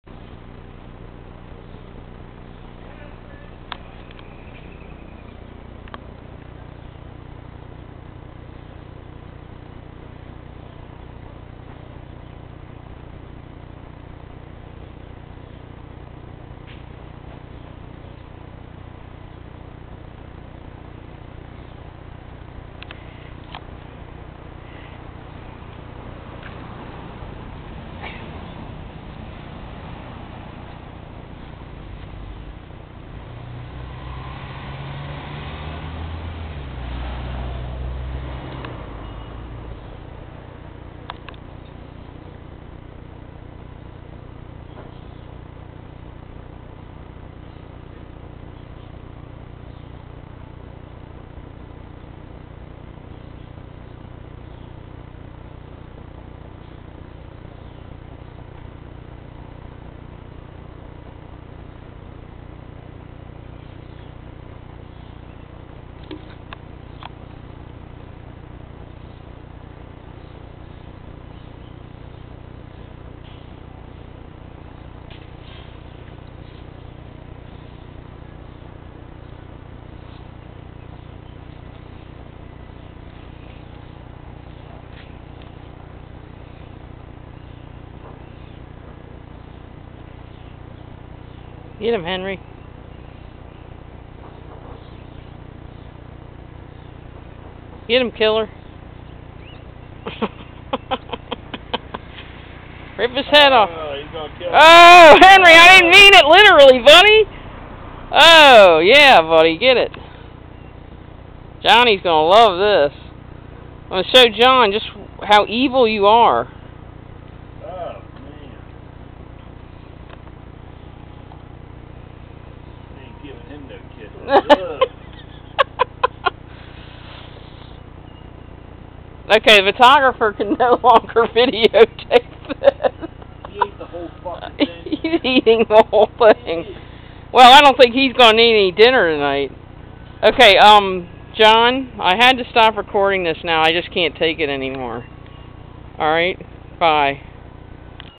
Henry the Killing Machine
Henry catches his prey (a mouse)